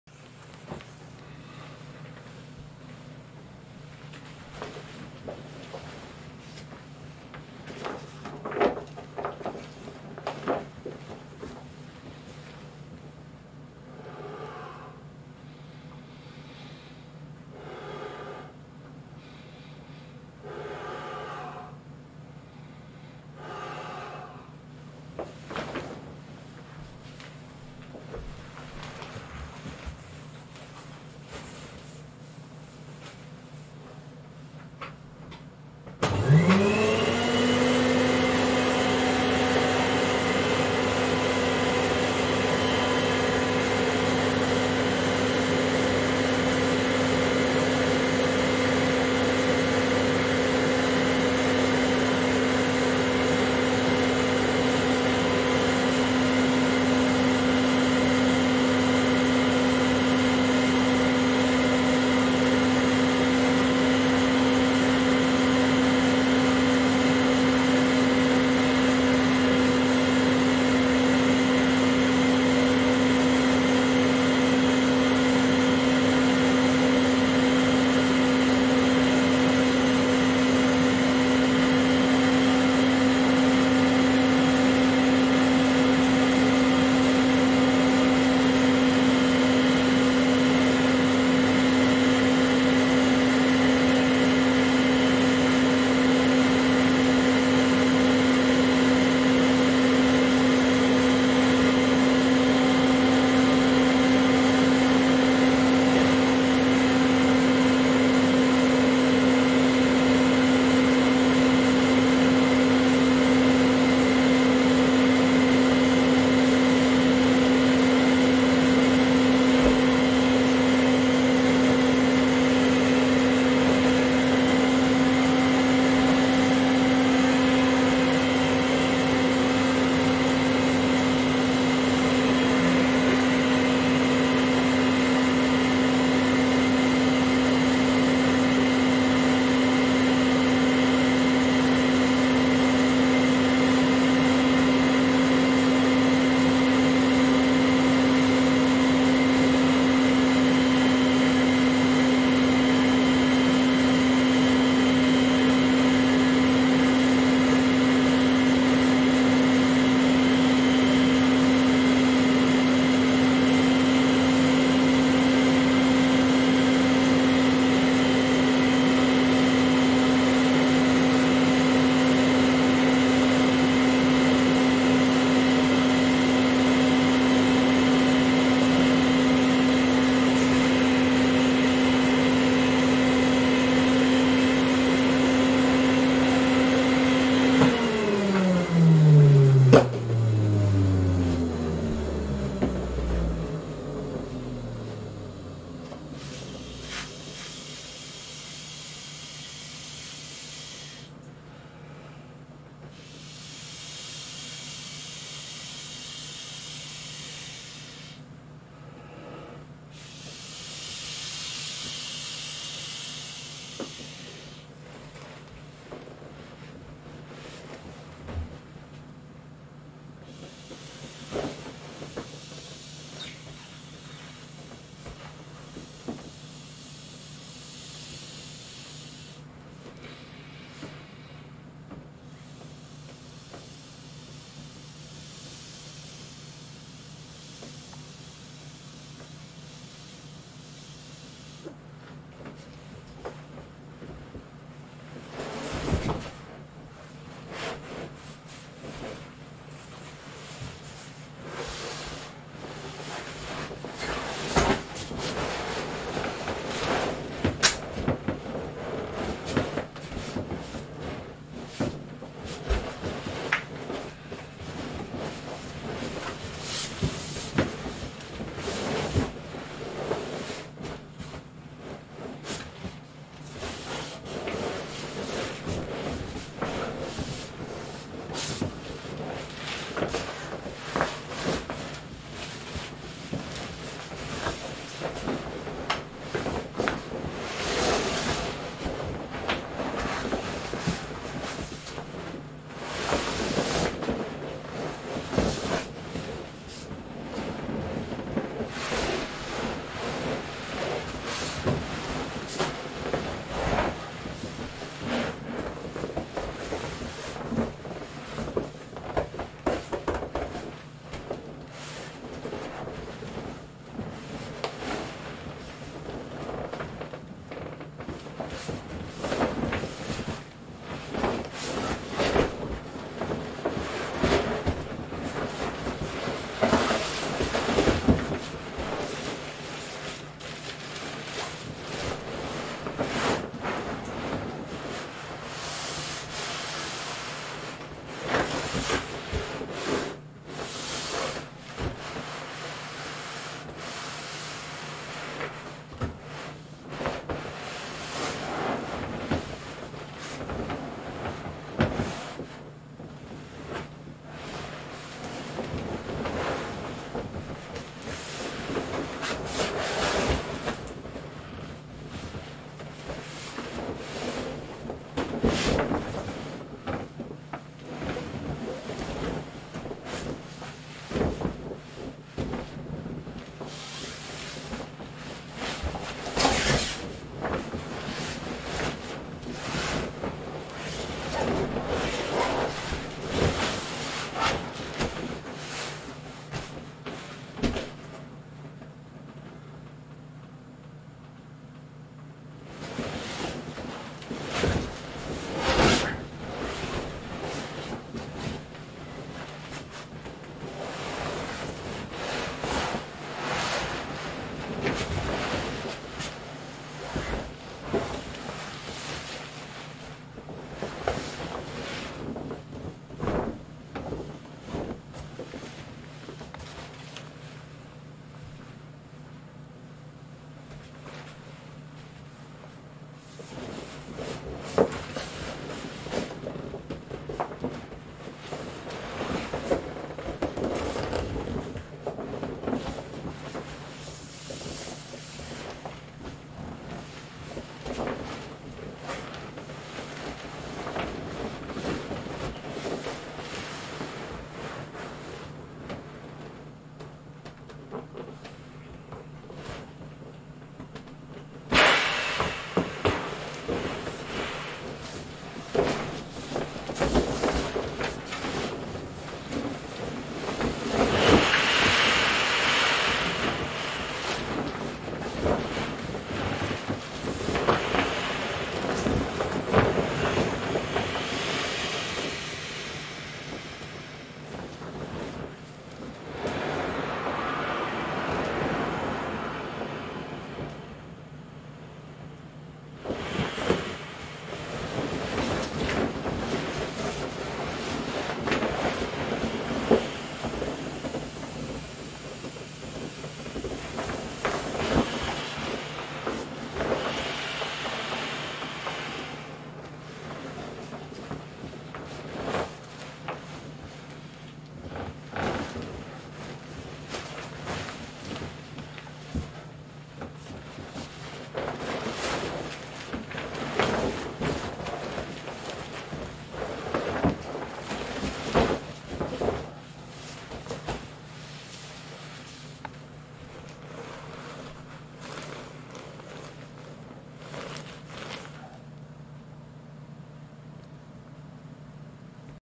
Big pool fast inflate and play
An old vacuum cleaner with an output air hose connector - it's a great inflating device for a big toys with a wide "rapid" or screw valves!